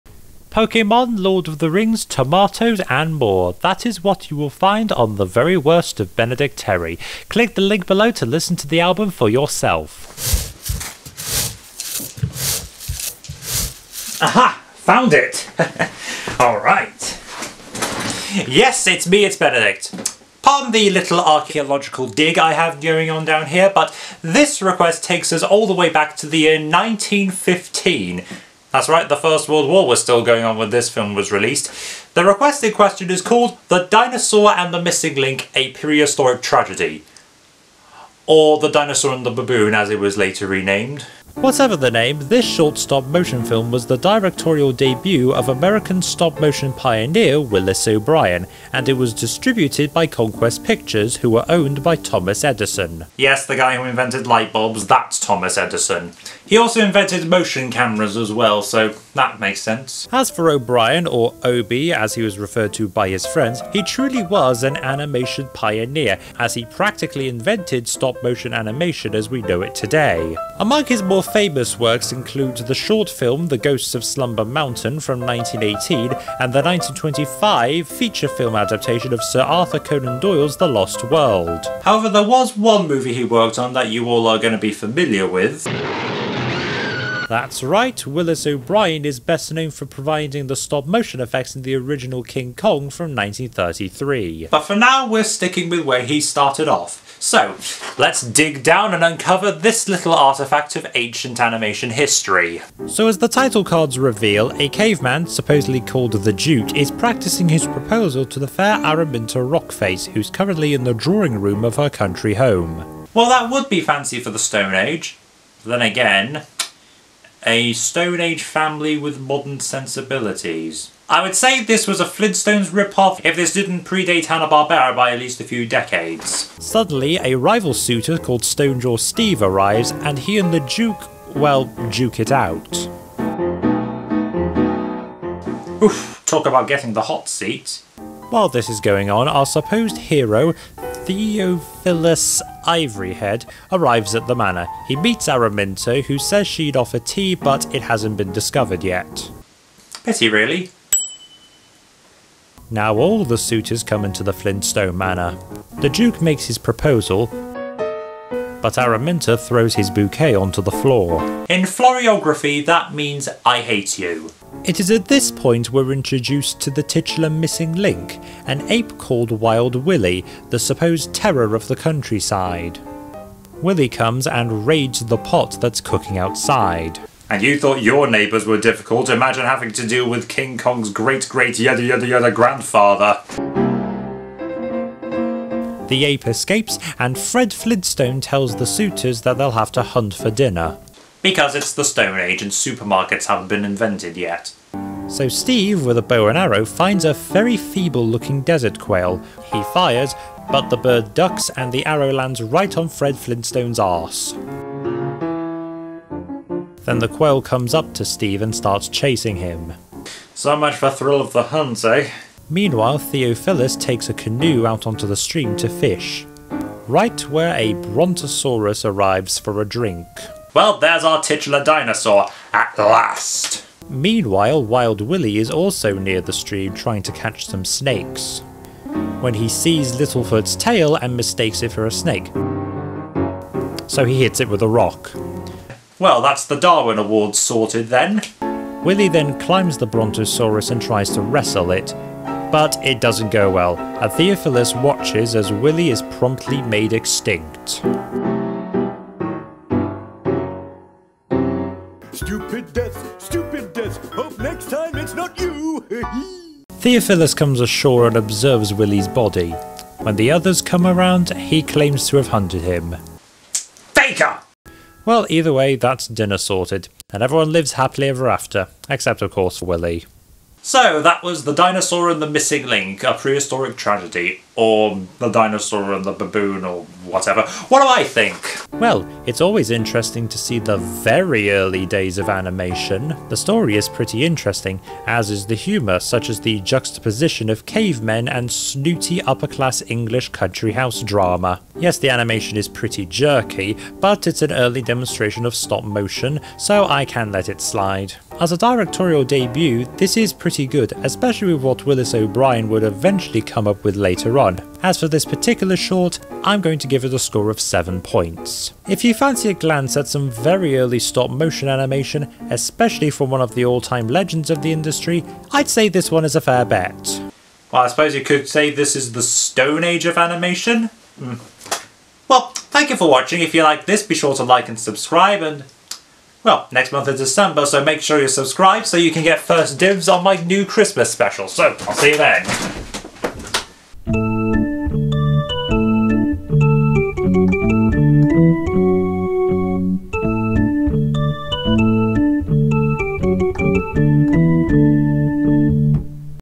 "The Dinosaur and the Missing Link"- Benedict reviews #35
Let us take a trip to the stone age... of animation.

Special thanks to HewyToonmore for requesting this one.